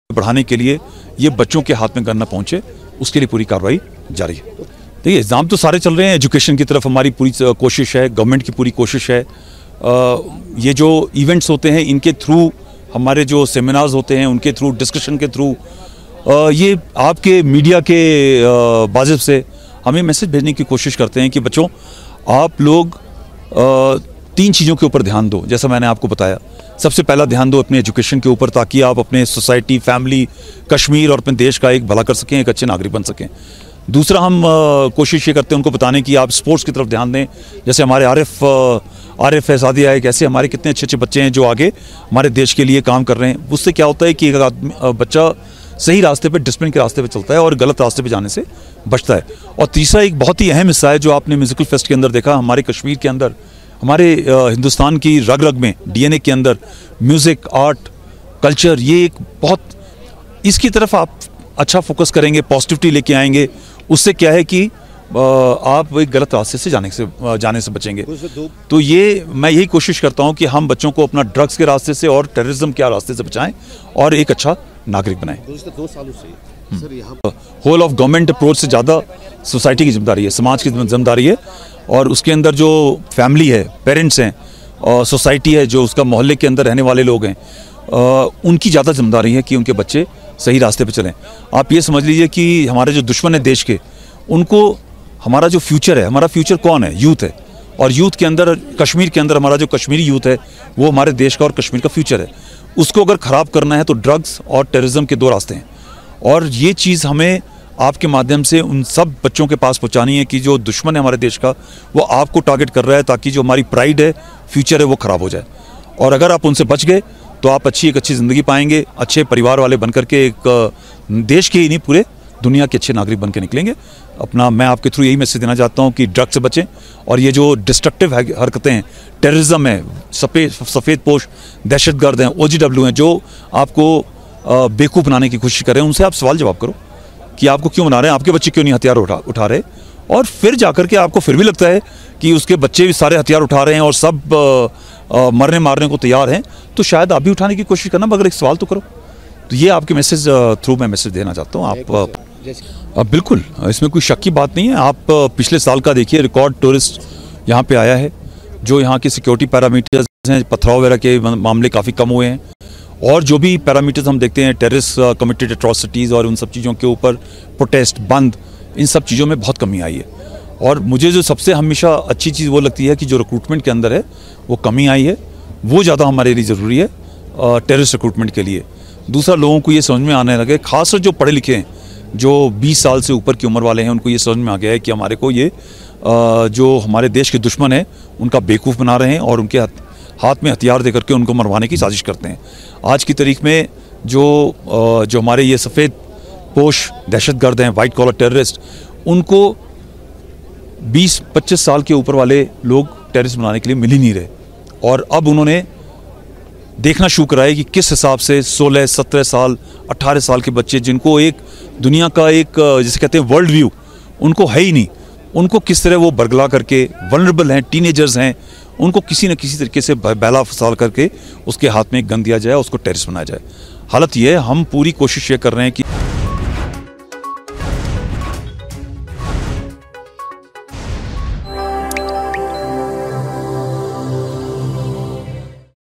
Indian Army Organizes Cultural Events for the Overall Betterment of the Students in J&K
Indian Army Organizes Cultural Events for the Overall Betterment of the Students in Jammu and Kashmir